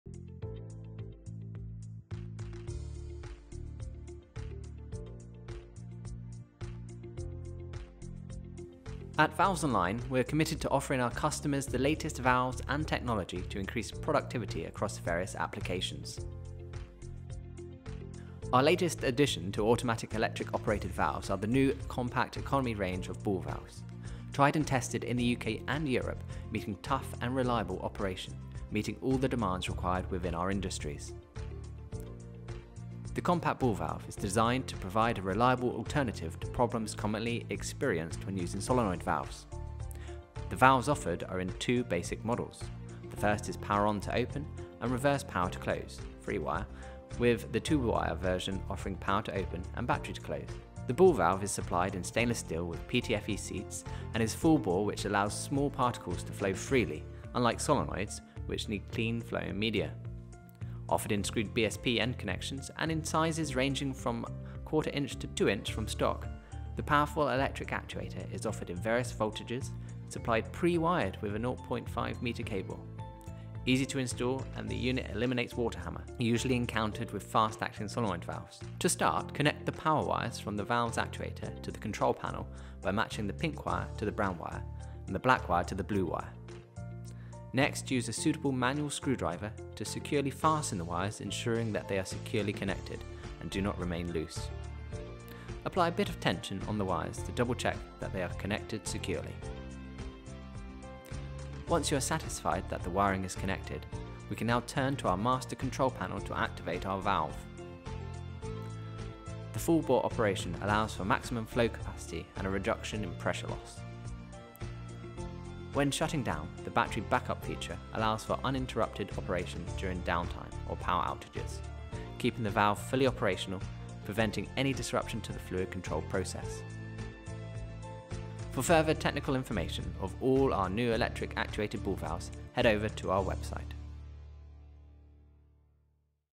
ELECTRIC ACTUATED BALL VALVE | The New Solution to the Solenoid Valve 😮
We run through a simple wiring tutorial on how to set up and activate the Electric Actuated Ball Valve to a master control panel.